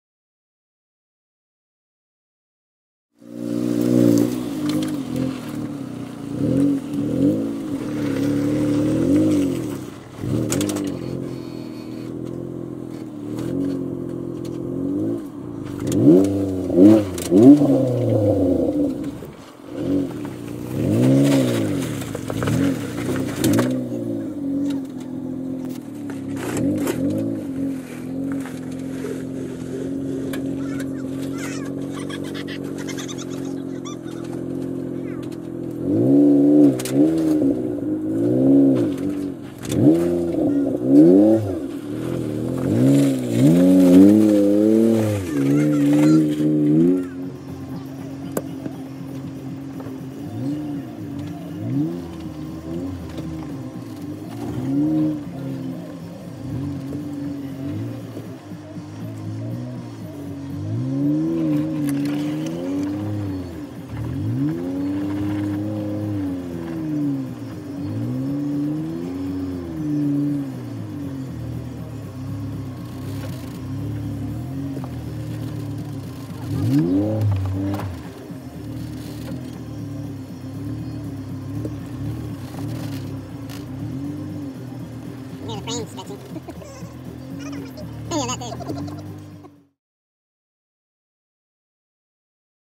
Charouleau Gap 6-2018
Off-road trail climbing Kissing Rock in Charouleau Gap in the Blue Burro